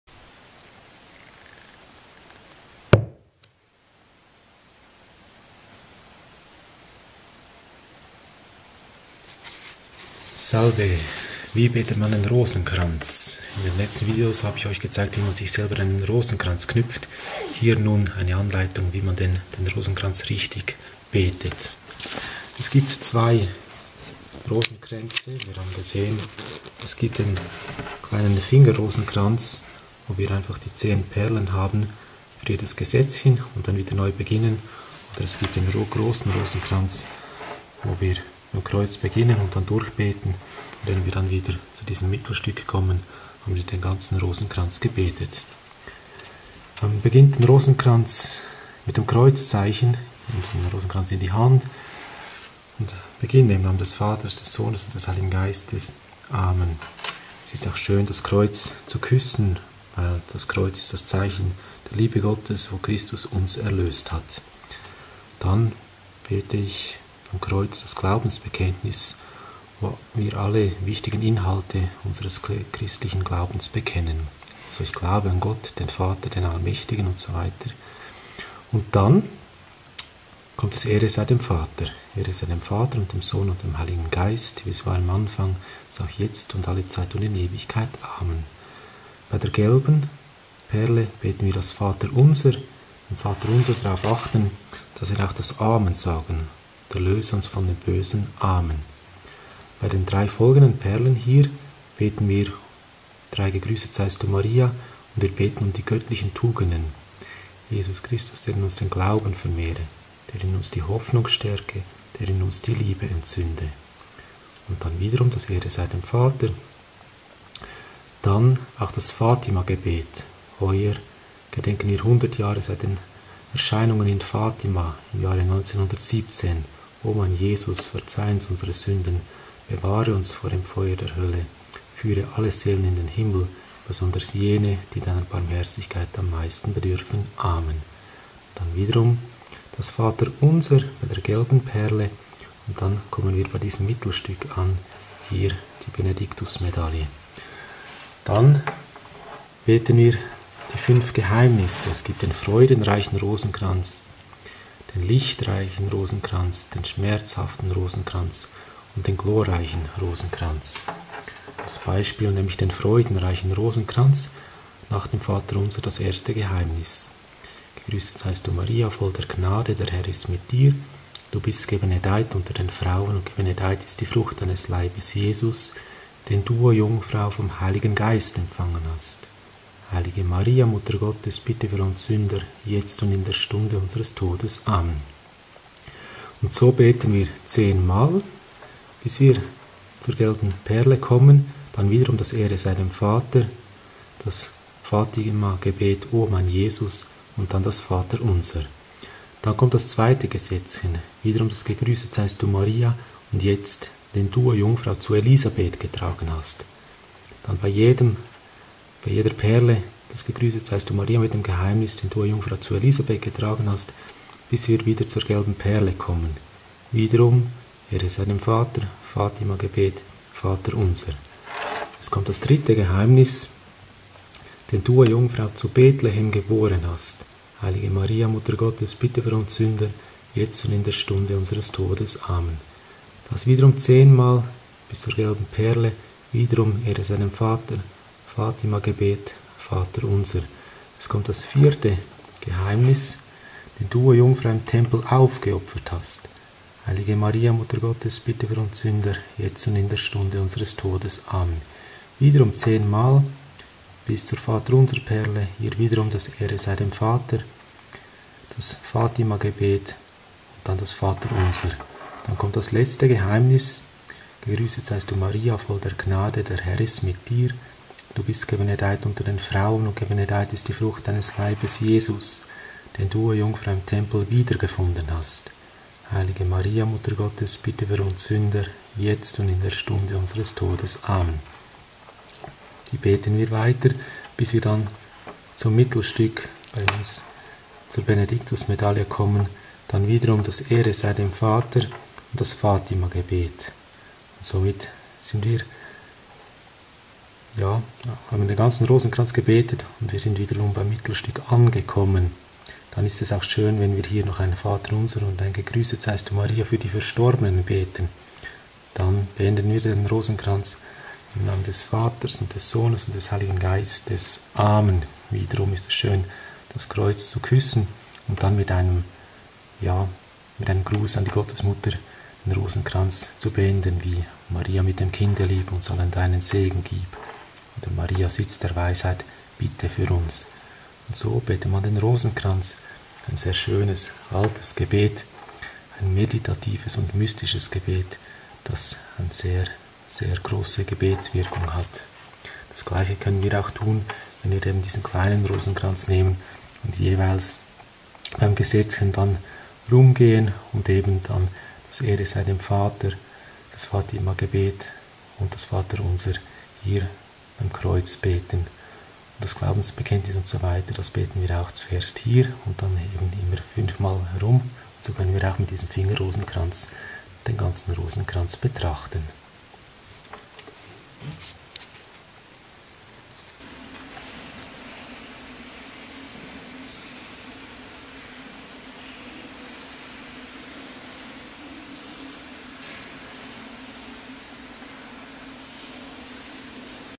Wie betet man den Rosenkranz?
Eine praktische Hinführung zum Rosenkranzgebet.